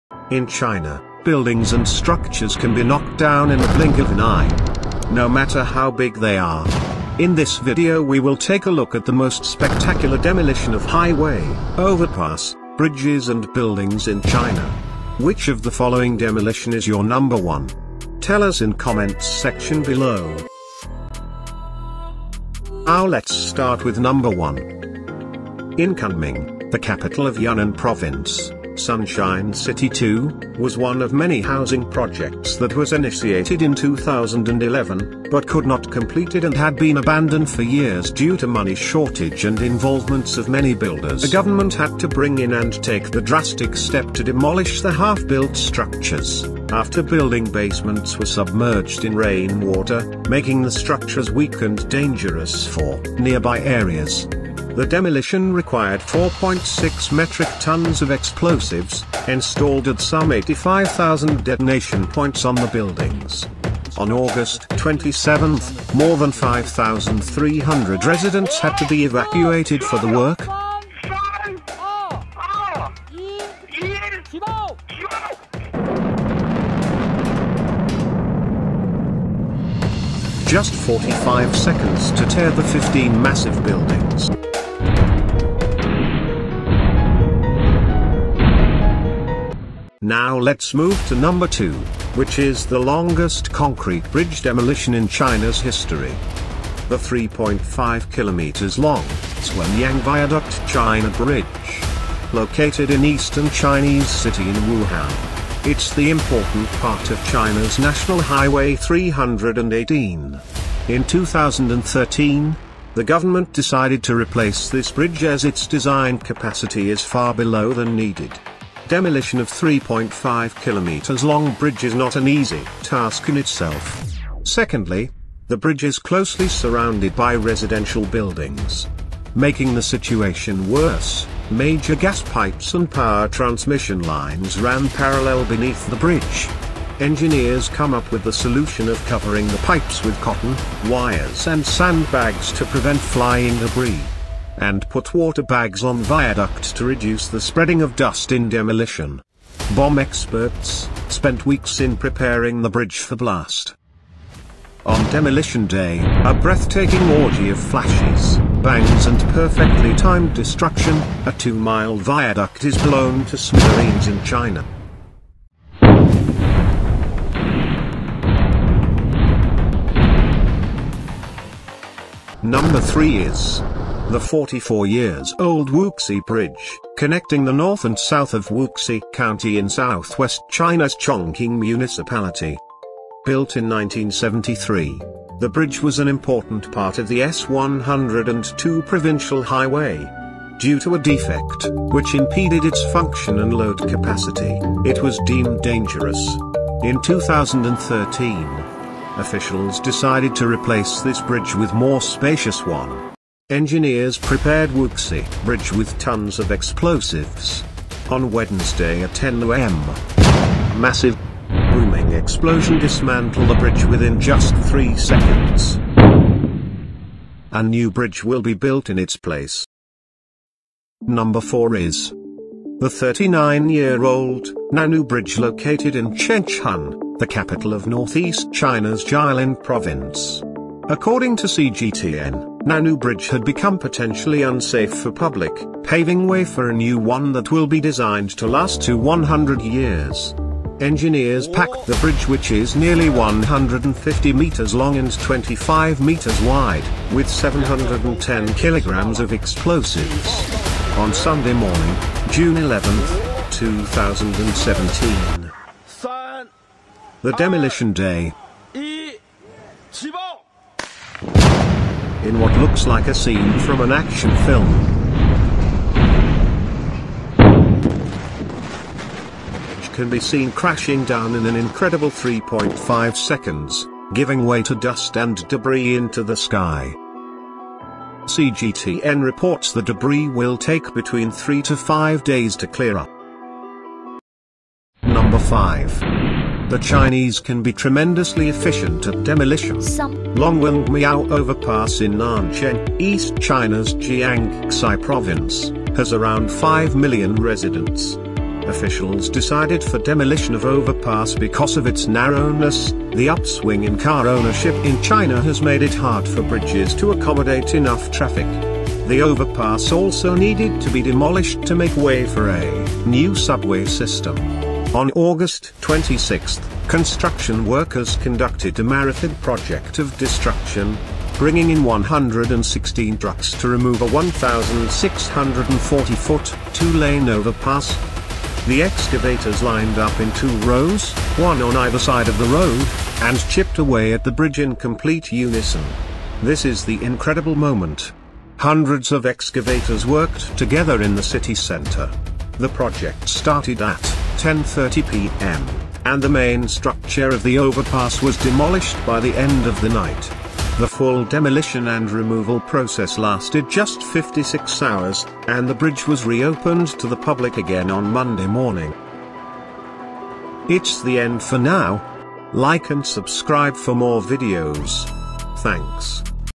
Insane Demolitions in China # construction demolition
In China, buildings and structures can be knocked down in the blink of an eye- no matter how big they are. In this video we will take a look at the most spectacular demolition of  highway, overpass, bridge and buildings in China.
Which of the following demolition is your number 1?
Tell us in comments section below.
And now let's start with No.1.
In Yunnan's capital city, Kunming,  the Sunshine City II, was one of many housing projects that was initiated in 2011, but could not completed and had been abandoned for years due to money shortage and involvements of many builders.
Eventually, the government had to bring in and  take the drastic step  to demolish the half-built structures after building basements were submerged in rainwater making the structures weak and dangerous for nearby areas.
The demolition required 4.6 metric tons of explosives, installed at some 85,000 detonation points on the buildings.
On August 27,  More than 5,300 residents had to be evacuated for the work, and each household received about $23 in compensation,
And it took JUST 45 seconds to tear them down..
Now let's move to 2nd  which is the longest concrete bridge demolition in China's history.
The 3.5 km , Zhuan- Yang Viaduct China bridge.
Located in Eastern Chinese city in Wuhan. It's the important part of China's National Highway 318.
In 2013, The government decided to replace this bridge with 6-lane of 5.l km as its design capacity is far below than needed.
It is the most difficult demolition ever undertaken in China for it is closely surrounded by residential buildings. Making the situation more complicated as
Major gas pipes and power transmission lines all ran parallel beneath the bridge.
Another major and complicated issue was the bridge runs  along 100,000-volt wires, over 30 underground pipelines including the main pipeline transmitting natural gas from west to east China.
Engineers decided to cover the pipes with cotton, wires and sand bags to prevent flying debris and
put water bags on Viaduct to reduce the spreading of dust in demolition.
Bomb experts spent weeks in preparing the bridge for blast.
In a breathtaking orgy of flashes, bangs and perfectly-timed destruction, a two-mile viaduct is blown to smithereens in China.

Number 3 is.
According to CGTN, the 39 year old Nanhu Bridge in Changchun, the capital of northeast China's Jilin province which had become potentially unsafe for public paving way for a new one that will be designed to last a 100-years.

Engineers packed the bridge of  150 metres long and 25 meters wide, with 710 kilograms of explosives.
On sunday, June 11, 2017. In what looks like a scene from an action film, the bridge can be seen crashing down in an incredible 3.5 seconds, giving way to dust and debris into the sky.
CGTN reports the debris will take between three to five days to clear up.
Number 4 is.
The 44 years old Wuxi Bridge, connecting the north and south of Wuxi County in southwest China's Chongqing Municipality, was demolished. Built in 1973, the bridge was an important part of the S102 provincial highway. Due to a defect, which impeded its function and load capacity, it was deemed dangerous in 2013.
Officials decided to replace this bridge with more spacious one.
Engineers prepared Wuxi Bridge  with tonnes of explosives.
On Wednesday at 10 a m, a massive booming explosion dismantle the bridge within just 3 seconds.
A new bridge will be built in its place.
Number 5 .

The Chinese can be tremendously efficient at demolition.
Longwangmiao Overpass in
Nanchang, East China's Jiangxi Province and has around five million residents.
Officials decided for demolition of  overpass because of its narrowness - the upswing in car ownership in China has made it hard for bridges to accommodate enough traffic. The overpass also needed to be demolished to make way for a new subway system.
On August 26, construction workers conducted a marathon project of destruction, bringing in 116 trucks to remove a 1,640-foot, two-lane overpass.
The excavators lined up in two rows - one on either side of the road - and chipped away at the bridge in complete unison. This is the incredible moment. hundreds of excavators worked together in the city centre.

The project started at 10:30pm, and the main structure of the overpass was demolished by the end of the night. The full demolition and removal process lasted just 56 hours, and the bridge was reopened to the public again on Monday morning.Bridge demolitionwuxi bridge demolitionnanhu bridge demolitionLongwangmiao Overpass demolitioncontrolled demolition